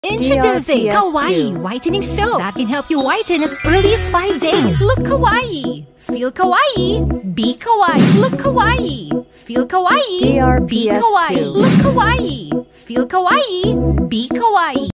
Kawaii Whitening Soap Radio Ad 2023 15s
Ito po ang 2023 Radio Ad ng Kawaii Whitening Soap. (15-seconder Version)

DRPS Two 4203: Kawaii Whitening Soap Radio Ad 2023 15s